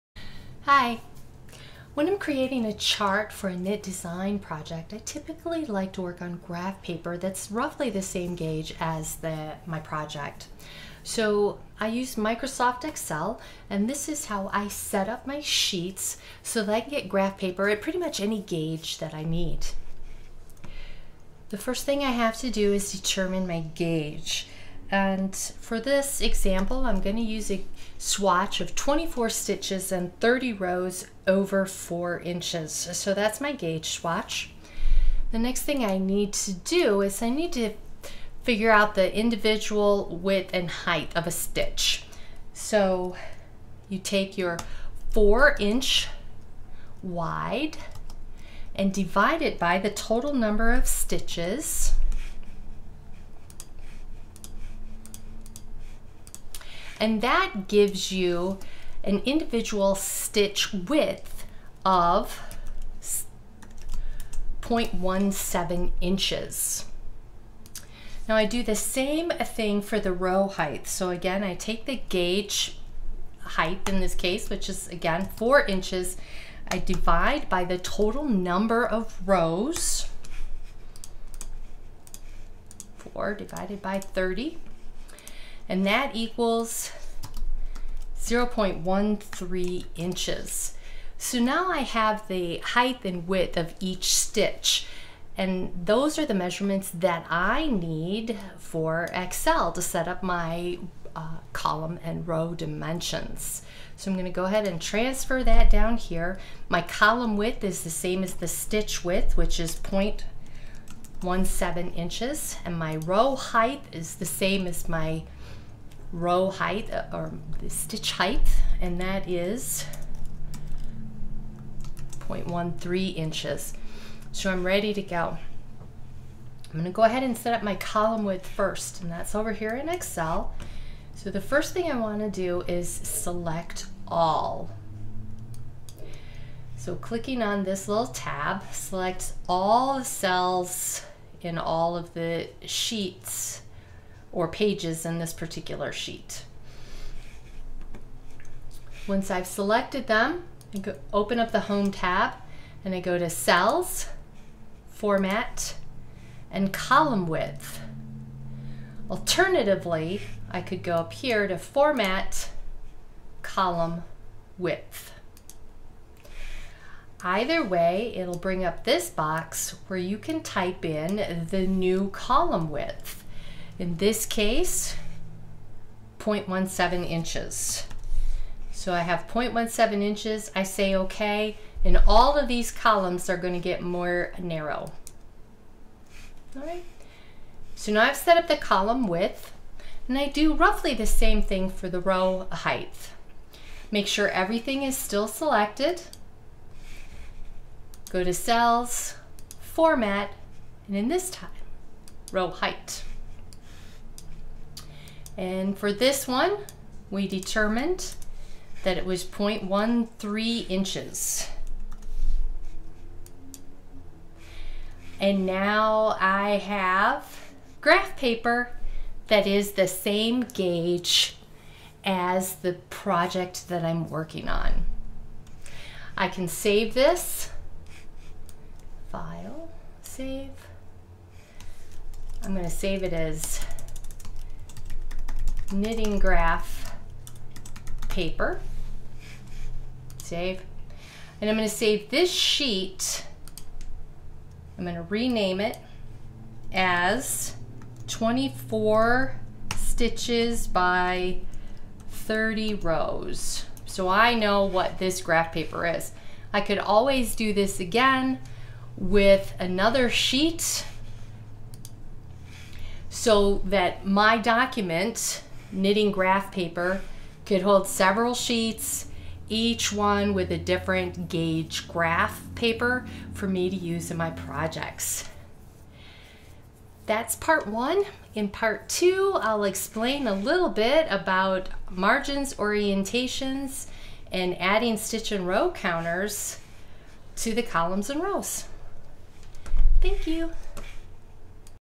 Knitting Graph Paper part1
How to set up MS Excel to create graph paper that matches your project gauge. In part 1, I show you how to go from stitch gauge to column and row dimensions in Excel.